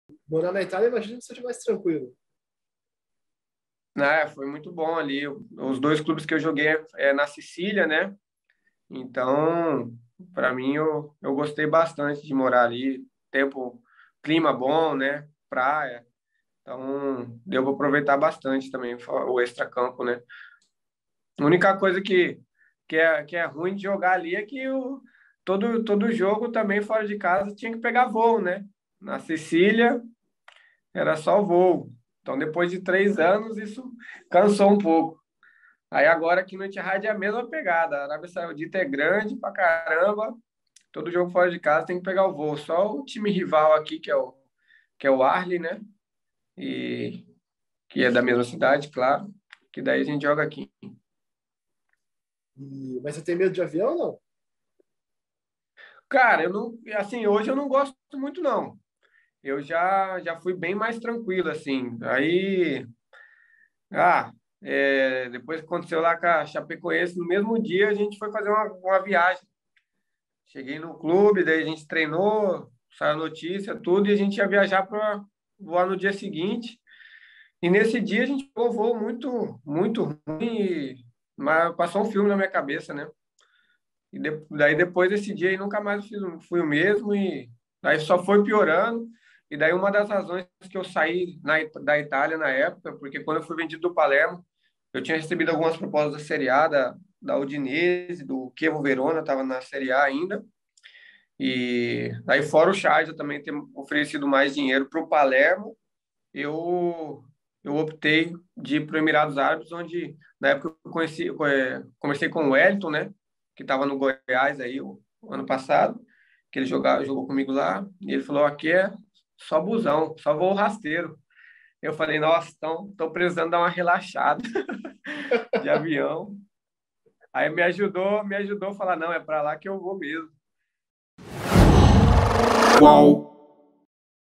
Igor Coronado, do Al-Ittihad, revela ter escolhido Emirados Árabes para fugir de avião
O meia brasileiro Igor Coronado, do Al-Ittihad (ARS), começou a carreira na Inglaterra, mas ganhou nome atuando no futebol italiano. O meia atuou por três anos em clubes da Sicília, ilha que pertence ao território italiano, e confessou em entrevista exclusiva ao UOL Esporte que se cansou de andar de avião. Esse foi um dos motivos que o fez escolher uma transferência para o futebol dos Emirados Árabes.